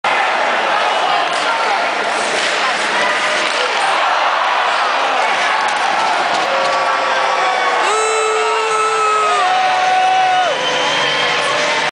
Alaska Aces at Victoria Salmon Kings 2 May 09
Aces 3rd goal by Josh Soares